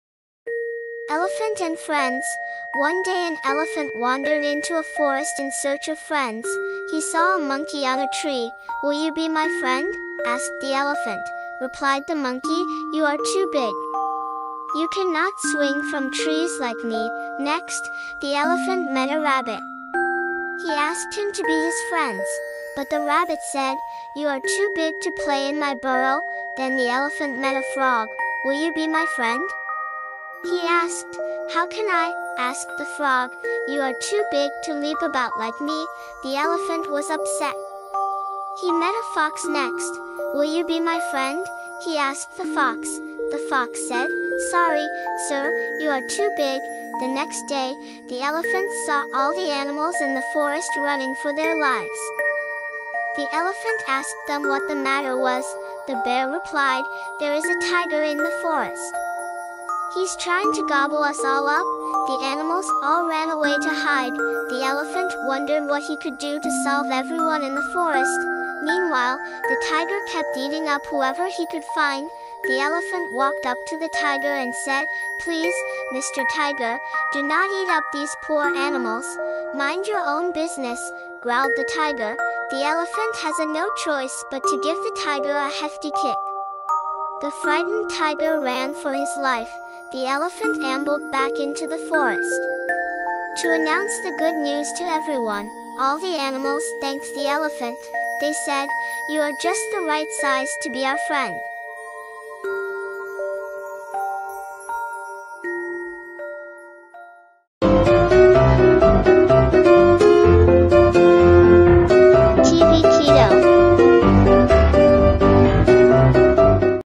Elephant and Friends
One day an elephant wandered into a forest in search of friends.
He saw a monkey on a tree.
"Will you be my friend?" asked the elephant.
Replied the monkey, "You are too big. You can not swing from trees like me."
Next, the elephant met a rabbit. He asked him to be his friends.
But the rabbit said, "You are too big to play in my burrow!"
Then the elephant met a frog.
"Will you be my friend? He asked.
"How can I?" asked the frog.
"You are too big to leap about like me."
The elephant was upset. He met a fox next.
"Will you be my friend?" he asked the fox.
The fox said, "Sorry, sir, you are too big."
The next day, the elephant saw all the animals in the forest running for their lives.
The elephant asked them what the matter was.
The bear replied, "There is a tiger in the forest. He's trying to gobble us all up!"
The animals all ran away to hide.
The elephant wondered what he could do to solve everyone in the forest.
Meanwhile, the tiger kept eating up whoever he could find.
The elephant walked up to the tiger and said, "Please, Mr. Tiger, do not eat up these poor animals."
"Mind your own business!" growled the tiger.
The elephant has a no choice but to give the tiger a hefty kick.
The frightened tiger ran for his life.
The elephant ambled back into the forest to announce the good news to everyone.
All the animals thanked the elephant.
They said, "You are just the right size to be our friend."